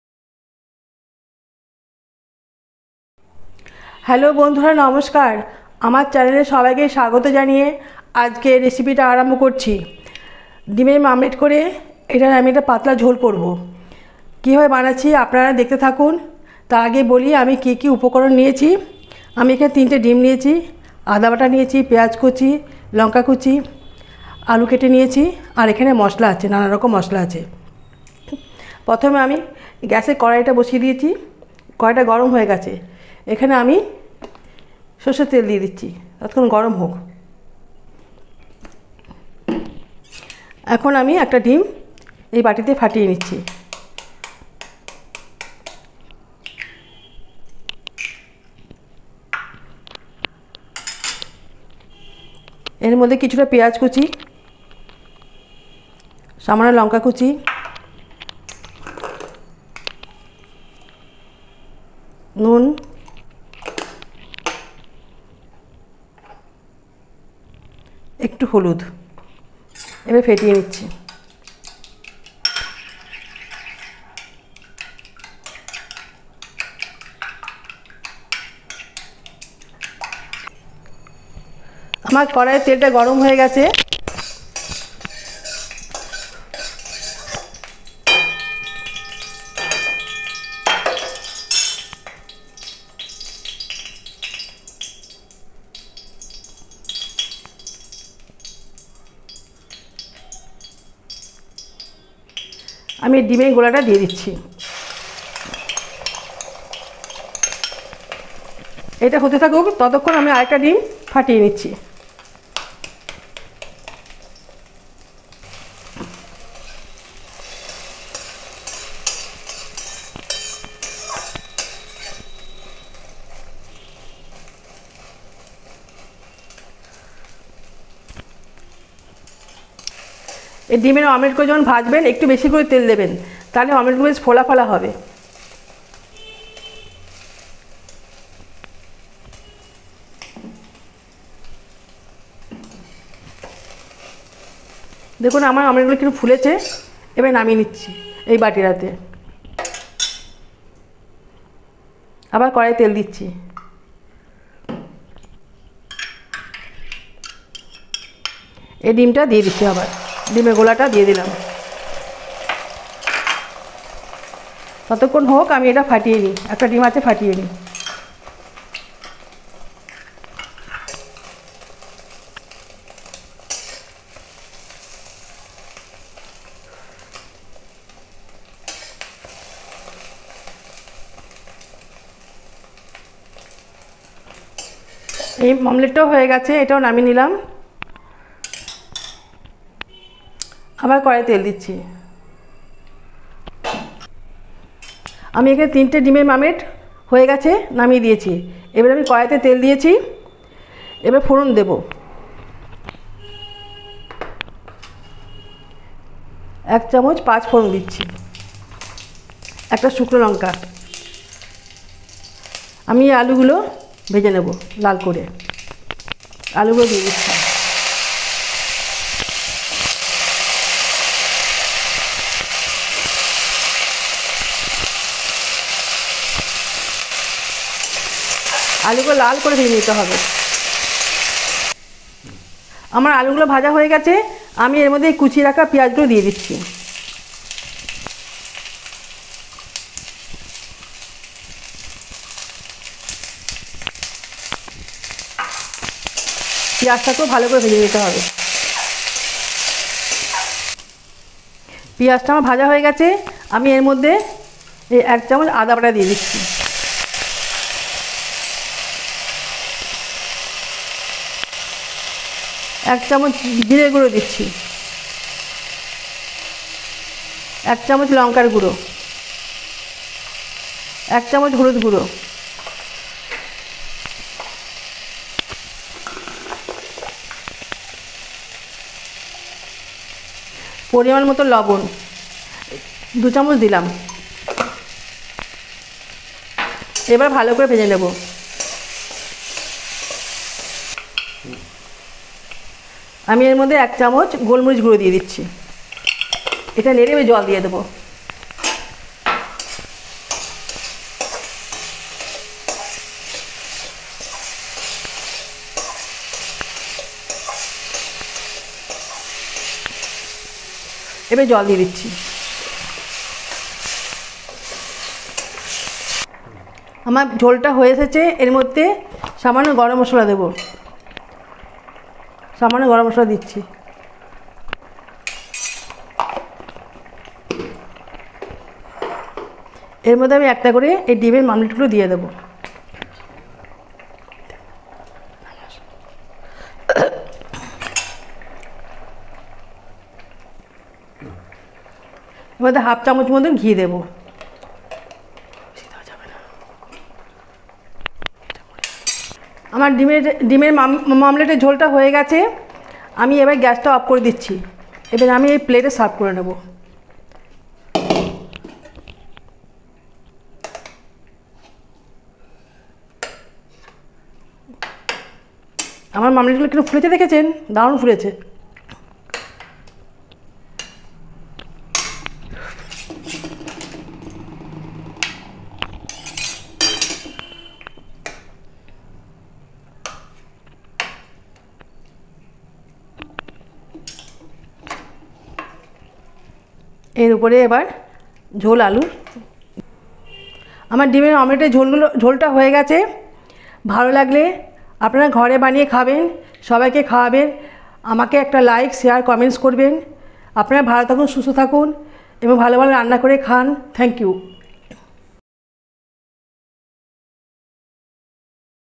Omelet Curry /Easy Lunch Recipe
Cooked by - Shila Das
Videography - Nakul Das

OMELET CURRY
Ingredients-
Egg , Onion , Salt , Cumin Seeds Powder , Garam masala , 5 Foron , Turmeric Powder , Ginger paste , Dried Chilies Powder , Dried Chili , Green Chili ,Mustard Oil , Clarified Butter , Pepper Corns ,